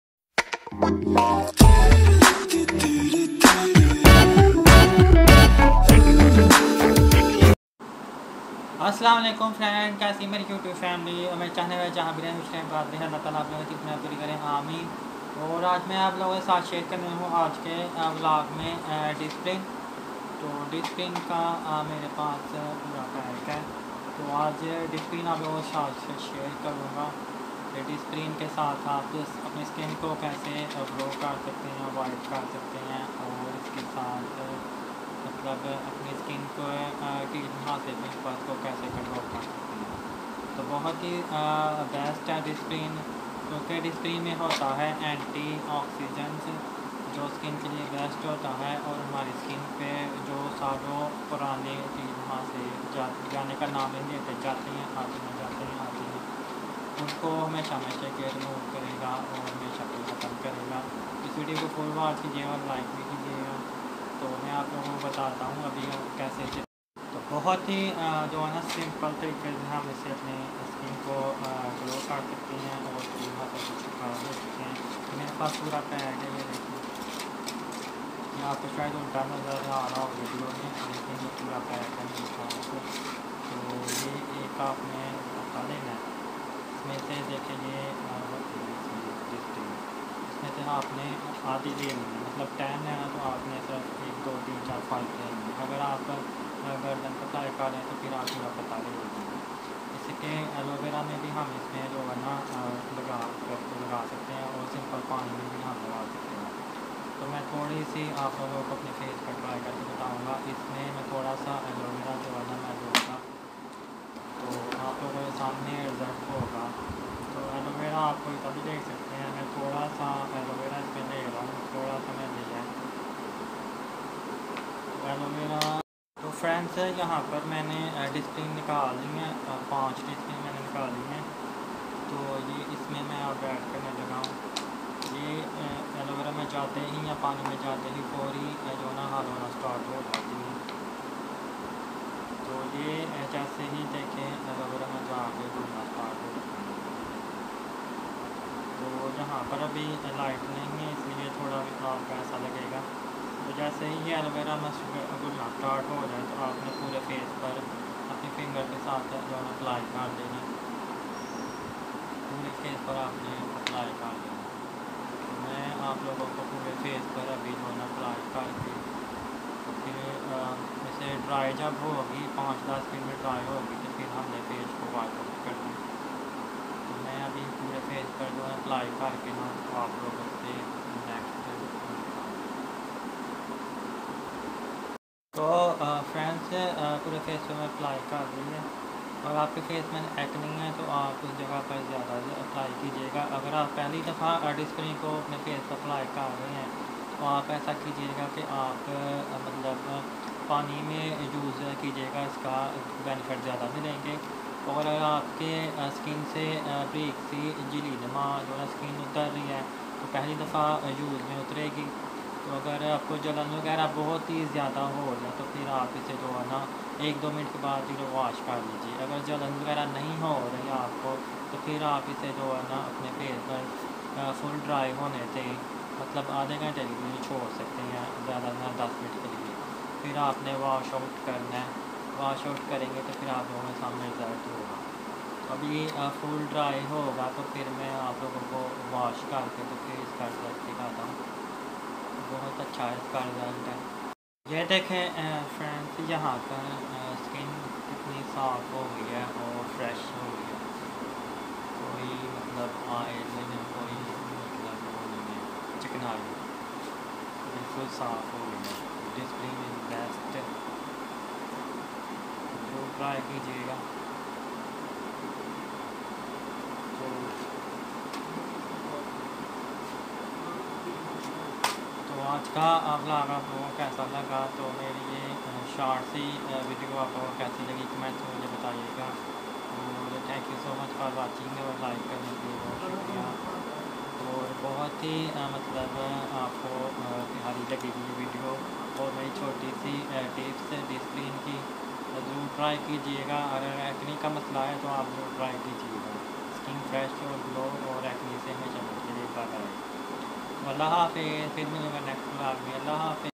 Disprin Tablets For Skin Freshness and Anti-acne /How To Use Disprin As Skin Care/Vlog
Disprin Tablets For Skin Freshness and Anti-acne /How To Use Disprin As Skin Care/Vlog#DisprinFaceMask# DisprinAsSkinCare#vlog